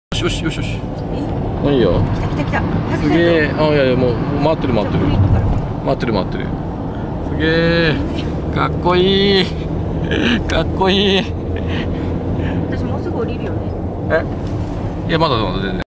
this bastard must be one of the fastest police car
that car is SUBARU Impreza WRX STi spec C which is the factory-made racing car for WRC. Hopefully this looks not a Impreza "S203" which is limited even-more-power-more-racing edition issued in 2005 because of different wheel design. This car has small flash lights added on the front face and tinted windows on the rear, as "Highway patrol police" spec, other looks pretty normal. but this particular car is not normal, though. original purpose of this model was to beat MITSUBISI Rancer EVO up in the rally field, but this time is not. not only Rancer but you unfortunately. if you had to drive on the freeway from Narita-international airport to Tokyo, must have driven safe and always watch out this BUSTARD, they might drive right behind you to bust you.